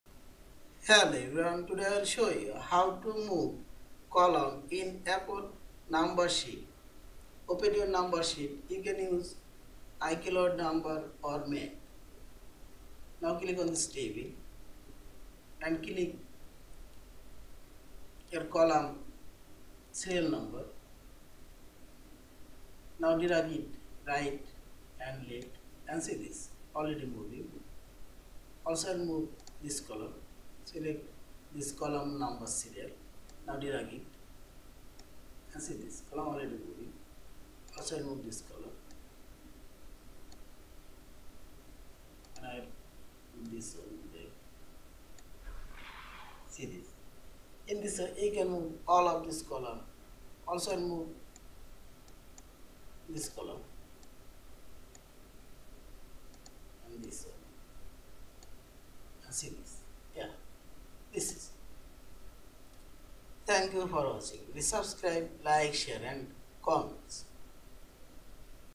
How to move columns in Apple Numbers (Spreadsheet) on Mac
Hello everyone, Today I will show you, How to move columns in Apple Numbers (Spreadsheet) on Mac.